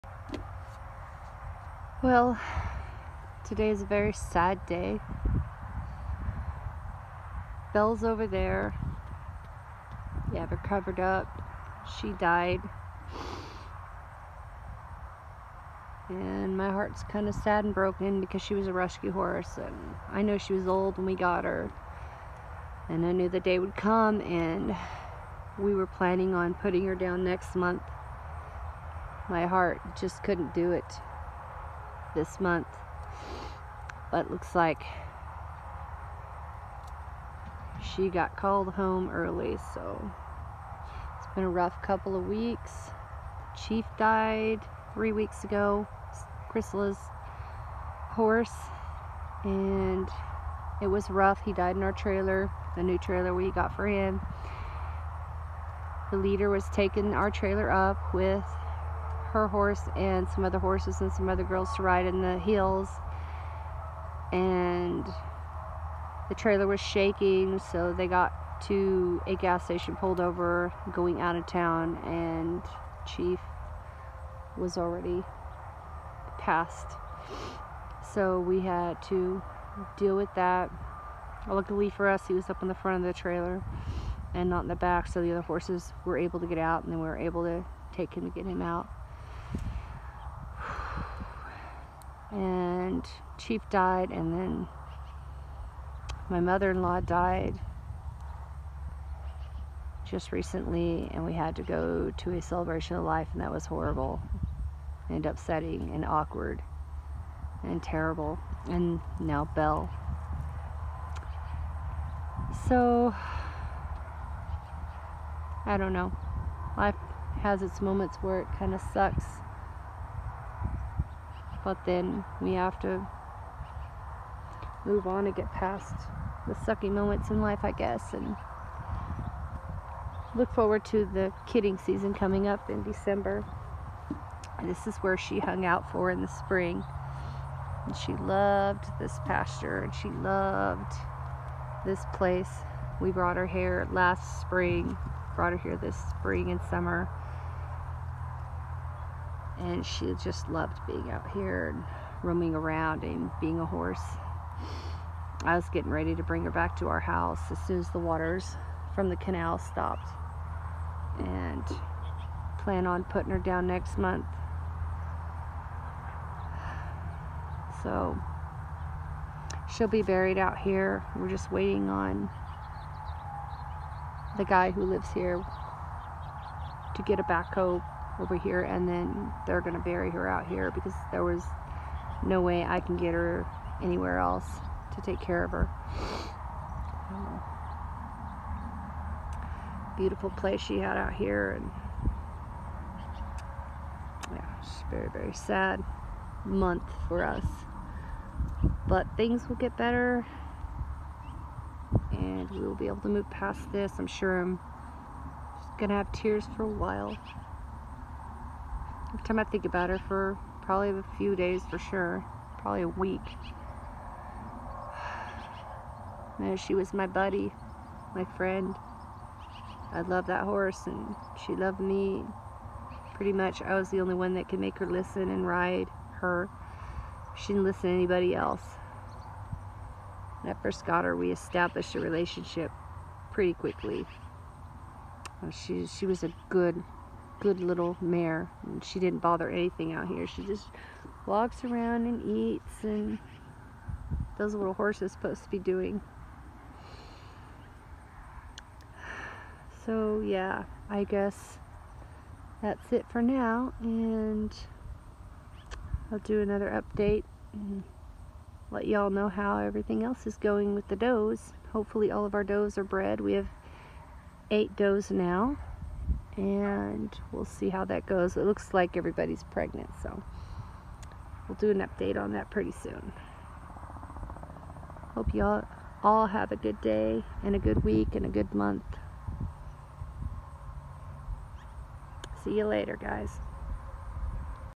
A Rough Month!
We lost Chief our 11yr olds gelding , my mother in law and Little Bell my old mare! God is good through good times and bad as well! I trust in Jesus !  Joshua 1:9 KJV ❤️ “Have not I commanded thee? Be strong and of a good courage; be not afraid, neither be thou dismayed: for the LORD thy God is with thee whithersoever thou goest.”